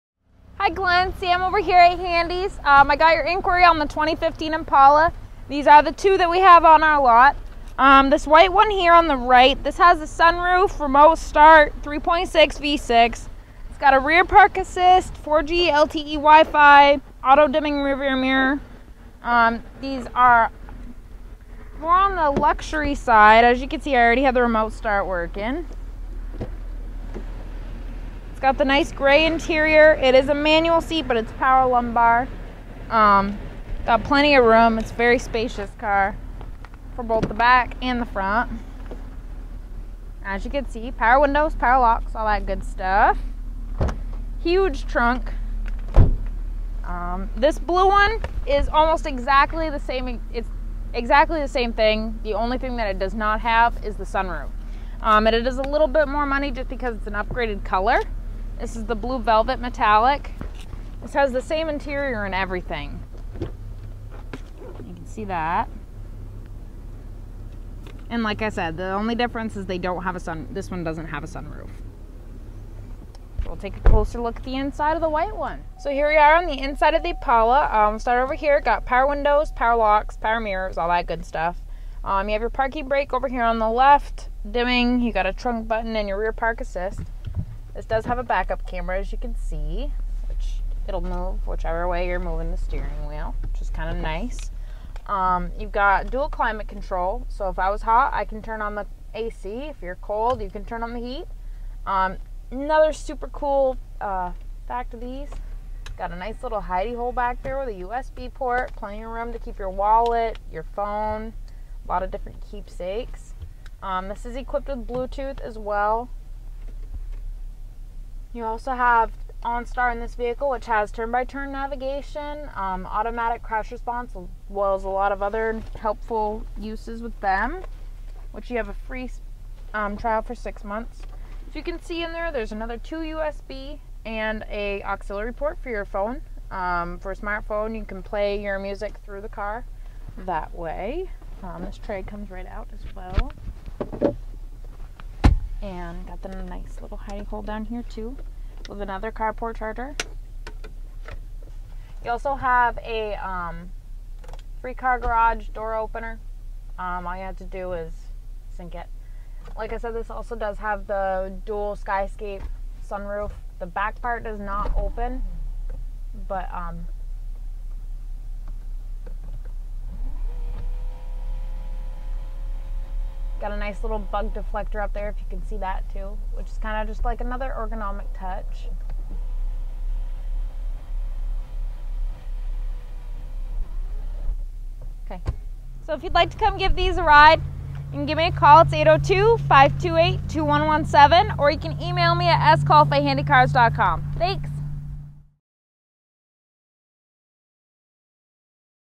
2015 Chevrolet Impala for Glen from Sam
Sam shows the 2015 Chevrolet Impala here on our lot at Handy Chevrolet in Saint Albans. This vehicle comes with hands free controls on the steering wheel, and a touch screen display. It also features heated seats, and a panoramic moon roof. Come to Handy Chevrolet today to see it for yourself!